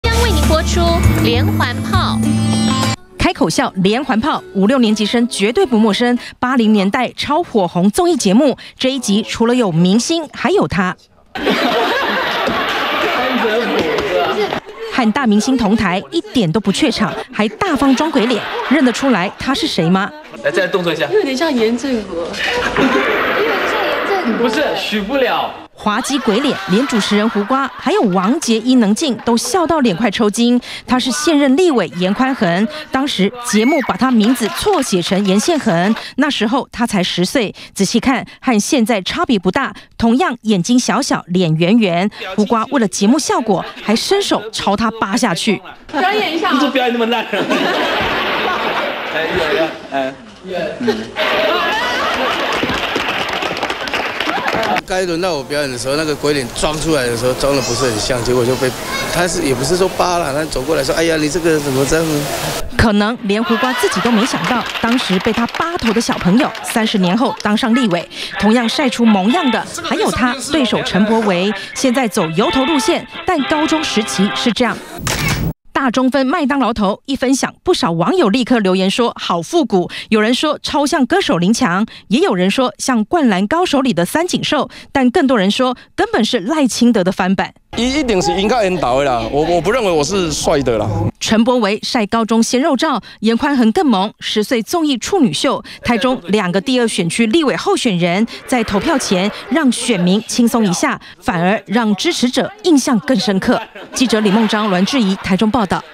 20200105中天新聞 10歲顏寬恒童顏曝光 上電視仿胡瓜還被巴頭
台中第二選區，現任立委顏寬恒對上代表綠營的陳柏惟，一個42歲，一個34歲，2個男人的戰爭，各有優勢，日前不約而同都PO出舊照分享，7年級生陳柏惟的高中照，頂著中分麥當勞頭，不少人說是賴清德翻版，而顏寬恒則是秀出小時候參加綜藝節目"連環泡"的萌照，當時才10歲，上節目和主持人胡瓜一起做鬼臉，超級可愛。